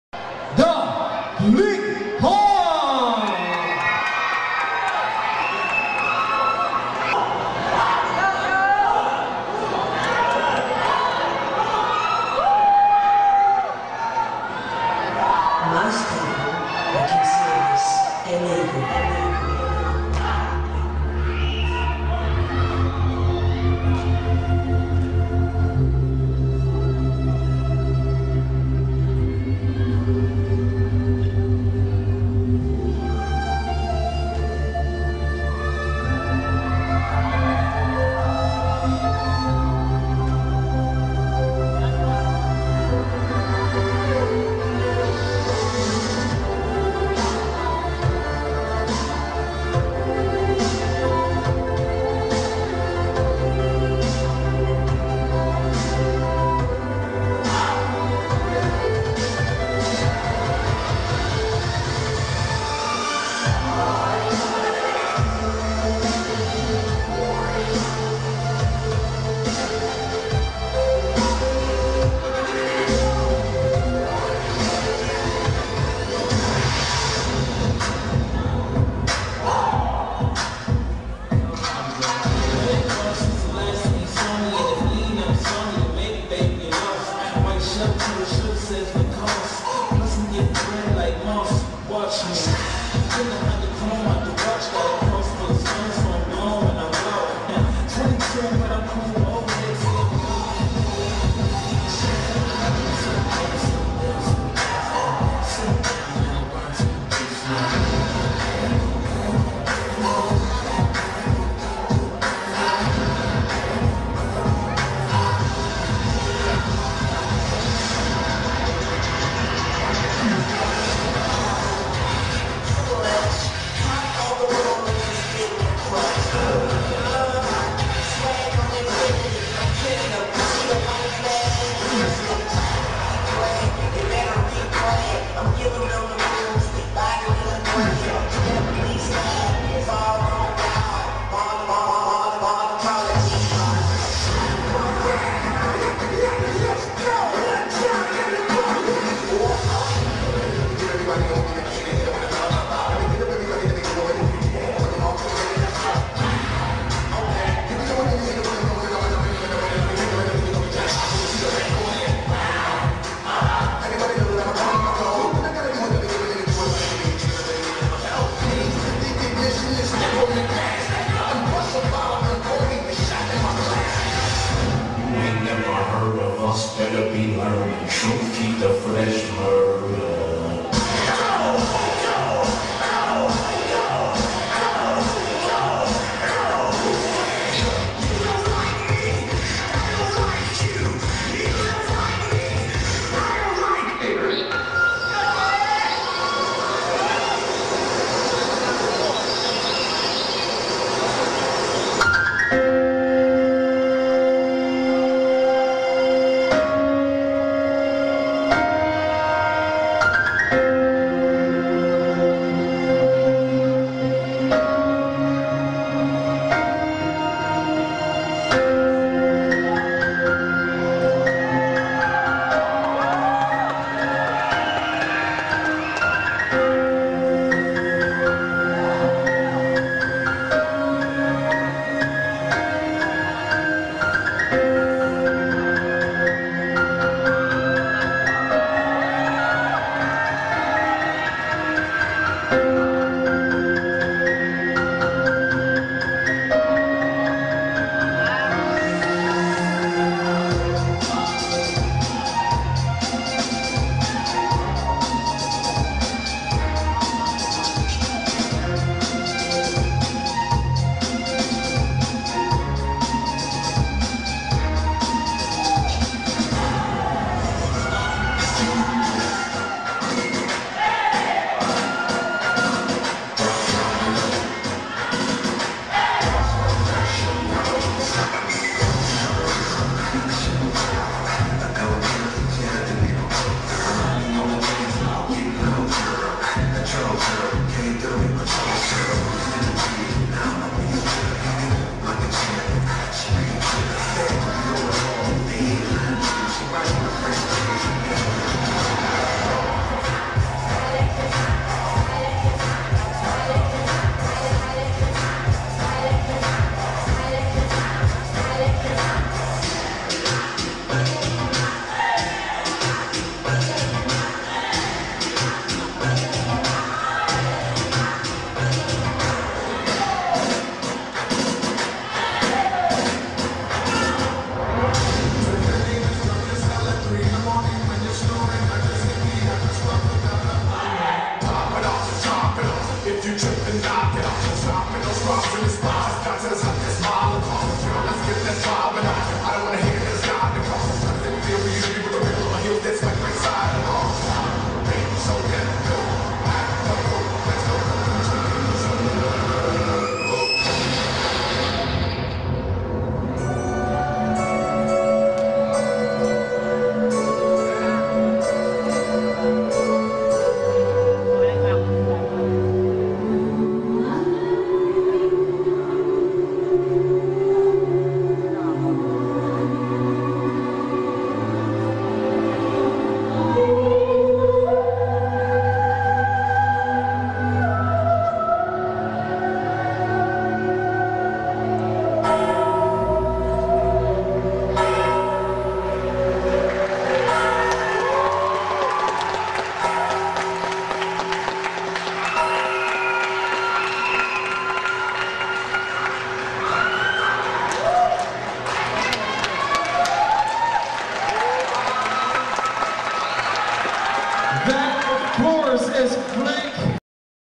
Flickhoundz/Sayaw Pilipinas Season 1
Sayaw Pilipinas Season 1 is a Dance Competition to be held at Mandaluyong City with our Production STAR APPLE PRODUCTION.

Event                                     Sayaw Pilipinas Season 1
Contingent Name                Flickhoundz Dancers
Production                           Star Apple Production and Talent Management Services  series of
                                               2010